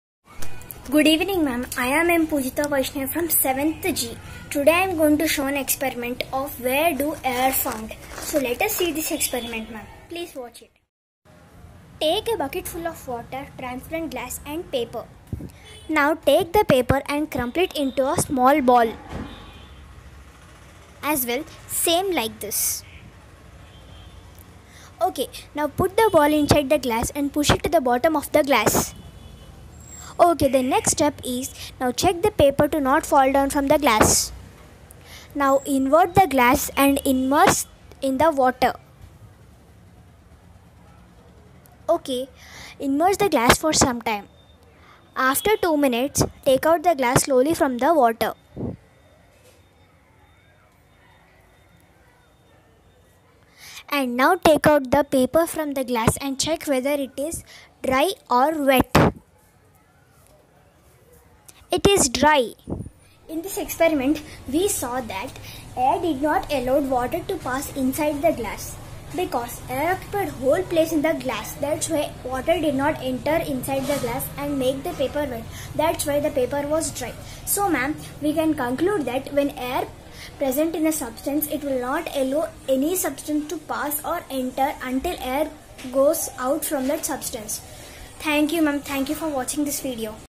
How to prove? - "AIR OCCUPIES SPACE"
One of my colleagues, Mr. Ramdas sir's daughter, explaining the topic with great conviction.